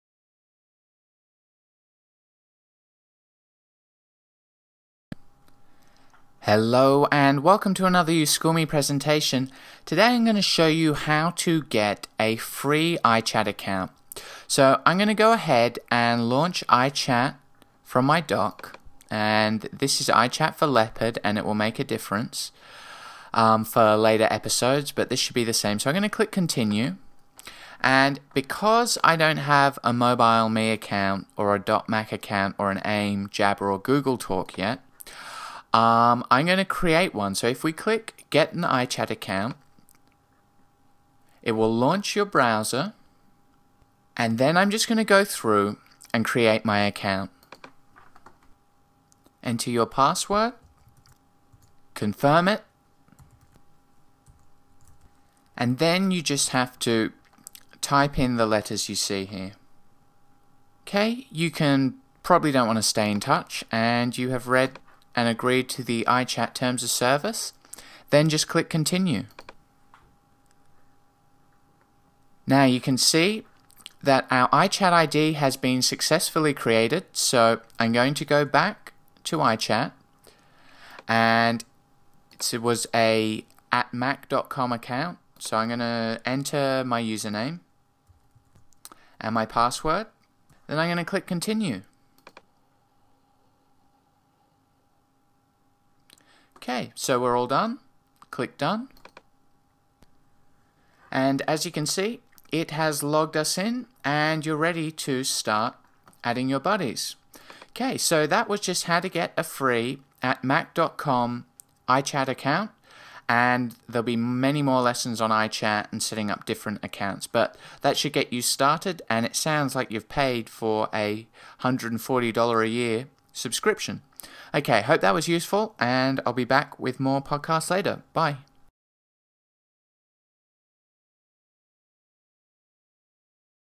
Learn iChat (Leopard): Lesson 1 - Creating a Free .Mac Chat Account @ uSchoolme
In this lesson you will learn how to create a free .Mac chat account with apple and set up this account in iChat for Leopard... then you can start adding your buddies and chatting, audio chatting, video chatting and more!!!